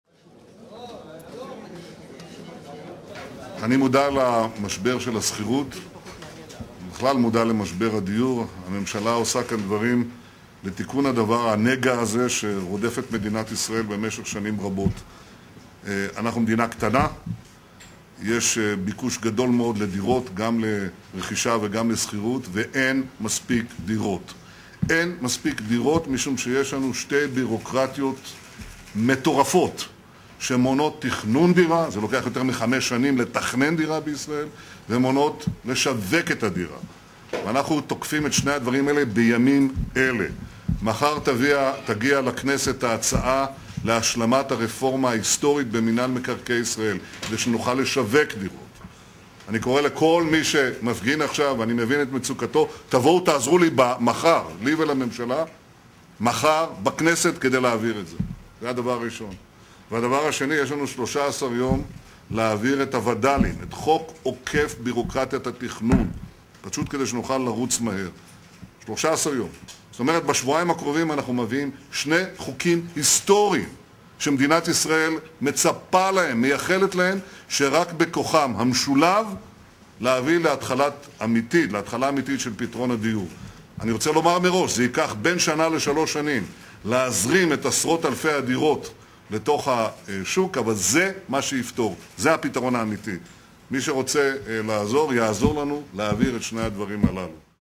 PM responds to tent protesters: Come protest in Jerusalem
Netanyahu says demonstrators should be working to advance his land reform proposal, housing and construction minister says plans to build apartments in periphery.

Subtitles and full article can be found on Jpost.com More from The Jerusalem Post